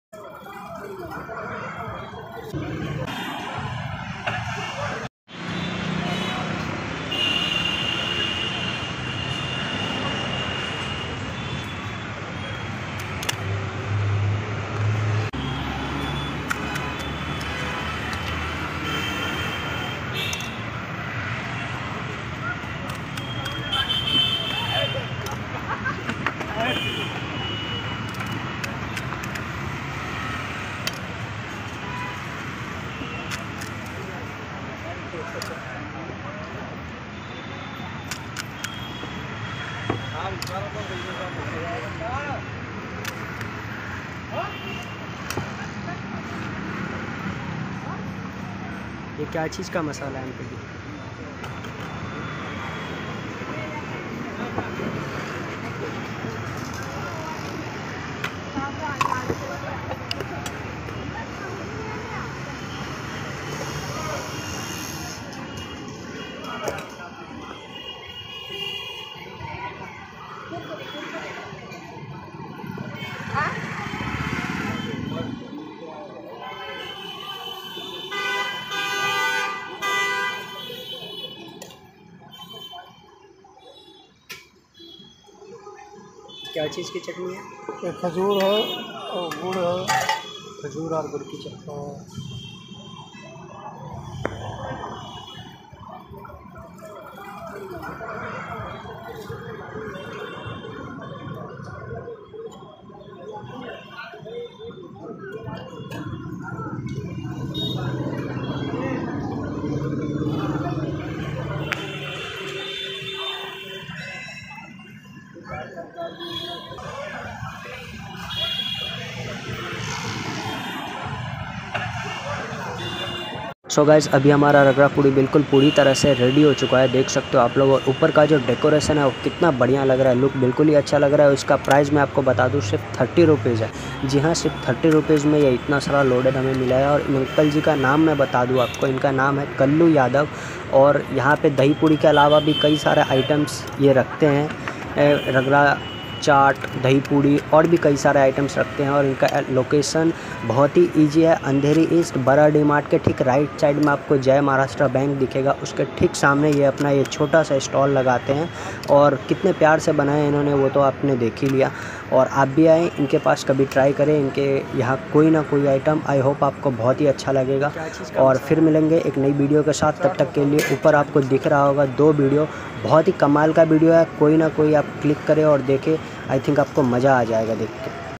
Famous Kallu Yadav Ji Ka Ragda Puri in Mumbai Rs.30/-Only |Ragda Puri Recipe| Indian Street Food
Watch this video to find out how to make Ragda Puri, a popular street food from Mumbai. This recipe is for making the dish at home and can be eaten with a variety of accompaniments such as chutneys and pickles.

Ragda Puri street food
Ragda puri recipe
Ragda puri Mumbai Street Food
Ragda puri in Mumbai
Famous ragda Puri street food Mumbai

Name: Kallu Yadav Ragda Puri Wala
Address: Andheri East, Mumbai
Opp, D- Mart, Jay Maharashtra Bank, ke Samne